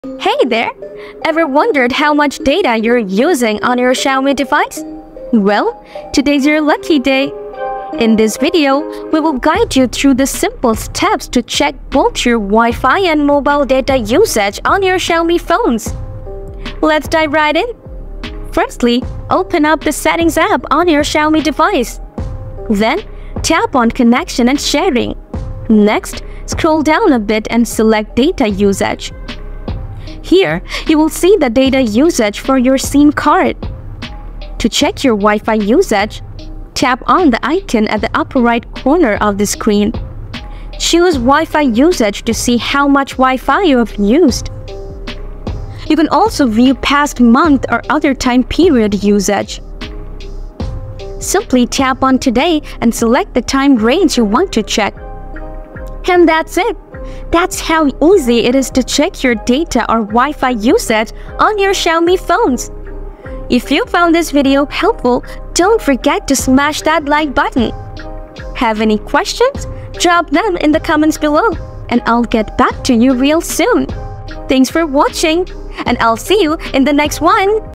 How to Check Wi-Fi and Mobile Data Usage on Xiaomi Phones
Wanna check how much data you've used on Mobile or Wi-Fi? If you need to find Wi-Fi data usage on Android, no need to worry! I'm here with a solution video guide on how to know Wi-Fi usage on Android and how to check Wi-Fi data usage on Xiaomi. Watch the video guide and discover how to check Wi-Fi data usage on Android.

In this quick and advanced video tutorial, I will show you how to check daily Mobile data and Wi-Fi usage on Android. So, watch the entire video and follow the instructions step by step carefully. After watching the video tutorial you will learn about Wi-Fi and Mobile data usage at Xiaomi and how to see Wi-Fi usage on Xiaomi.

00:01- Video intro
00:18- The Process
01:03- Process ending